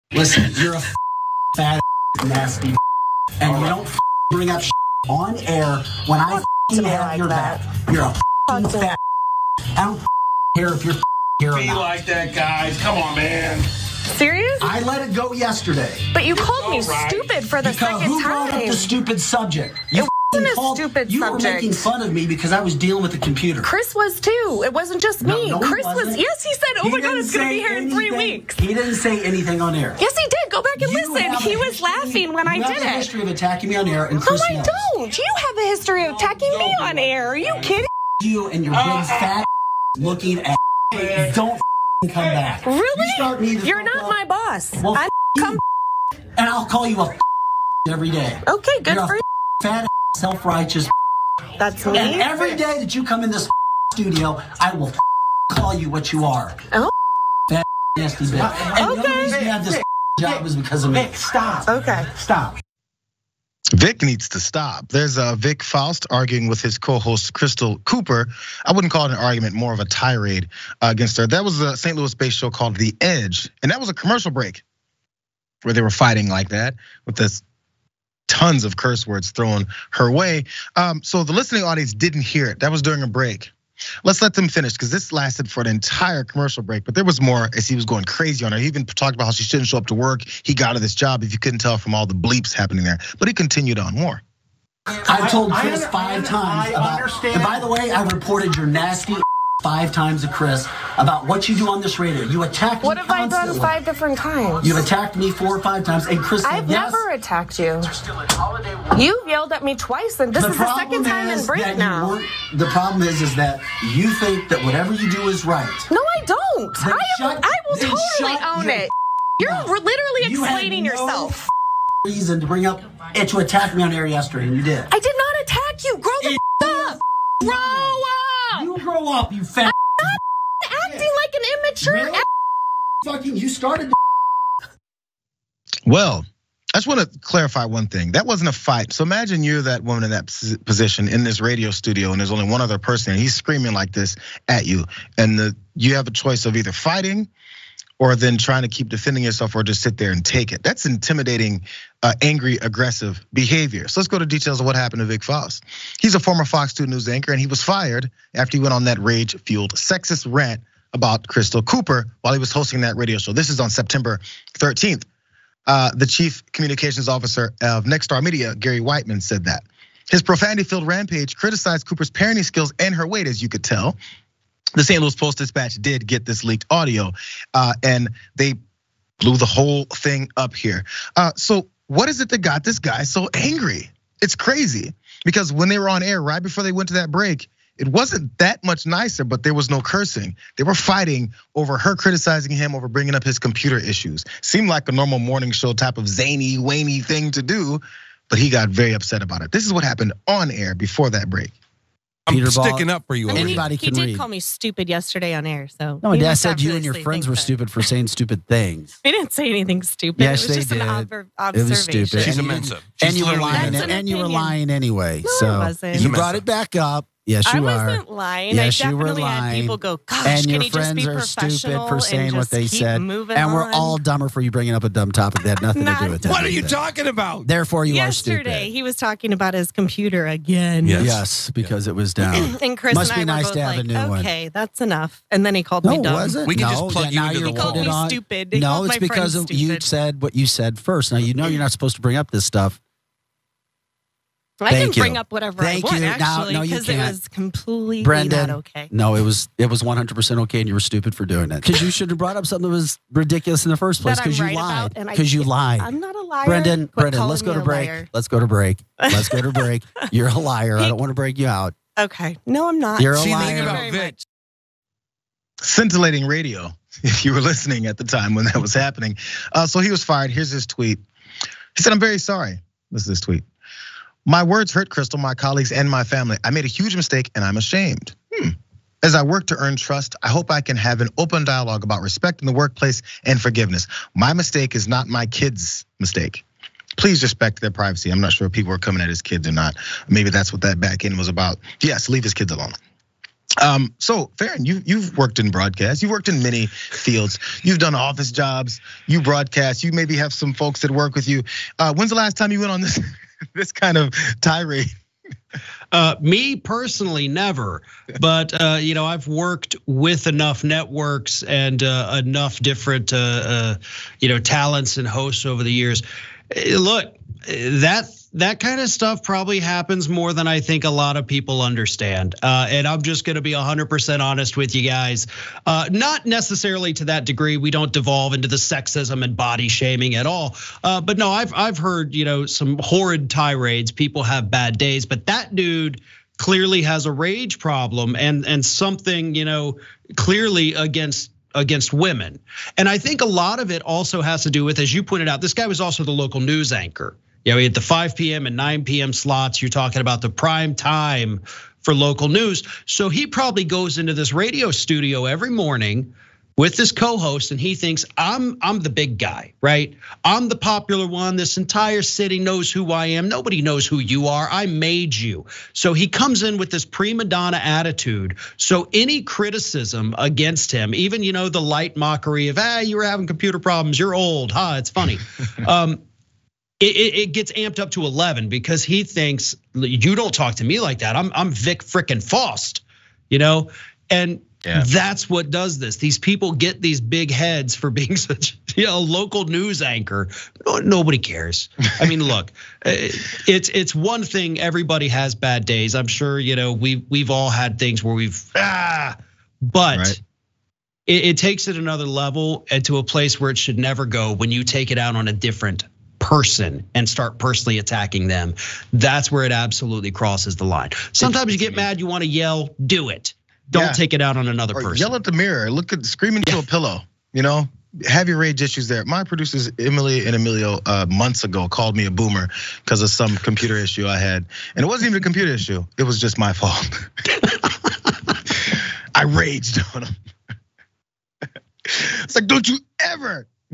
St. Louis Radio Host Fired After Explosive Misogynistic Rant
That morning radio team on “the edge” in St. Louis wasn’t having a very zany day last week when Vic Faust went on a curse-filled tirade towards his co-host Crystal Cooper. Jayar Jackson and Farron Cousins break it down on The Watchlist.

"Vic Faust no longer works for Fox 2, KPLR (Channel 11) or Nexstar Media,” station general manager Kurt Krueger said Wednesday morning. Nexstar Media Group is Fox 2’s parent company.

Krueger declined to comment further, citing personnel restrictions.

A billboard shows the Fox 2 News team of Vic Faust (from left), Mandy Murphey and Shirley Washington, along Highway 40 (Interstate 64) at 14th Street in St. Louis, on Tuesday, Sept. 20, 2022.

Faust used profanity at least 40 times in berating Crystal Cooper, who was part of Faust’s morning-drive show Sept. 13 on KFNS (100.7 FM), also known as “The Viper.”"

220922__WL06_St_Louis_Radio_Host